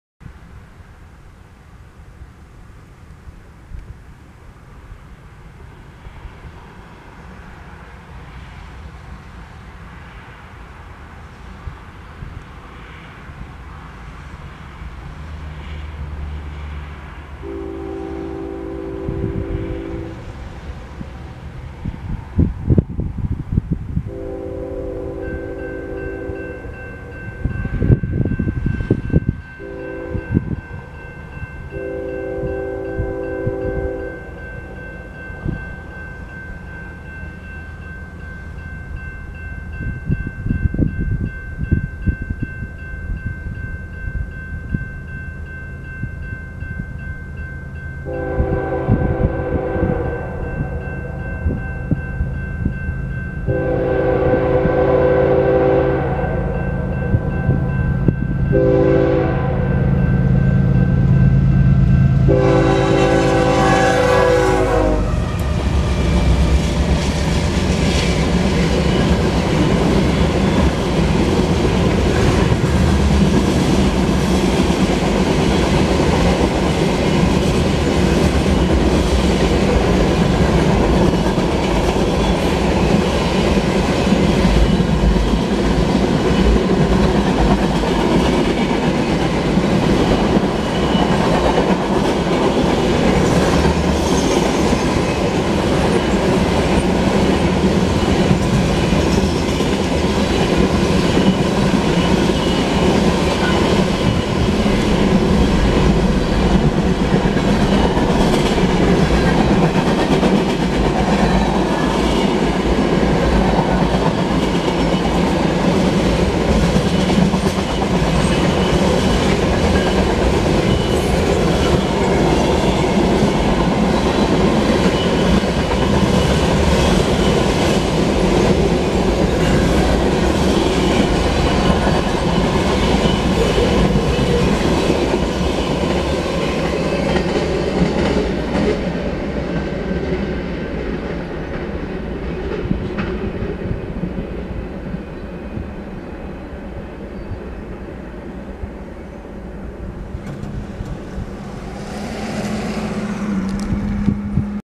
BNSF 6829 East Manifest Train (2-18-2017)
East Manifest Train/390 Axles/Main Track 2/M.P. 393.27
BNSF St. Croix Sub/11:33 AM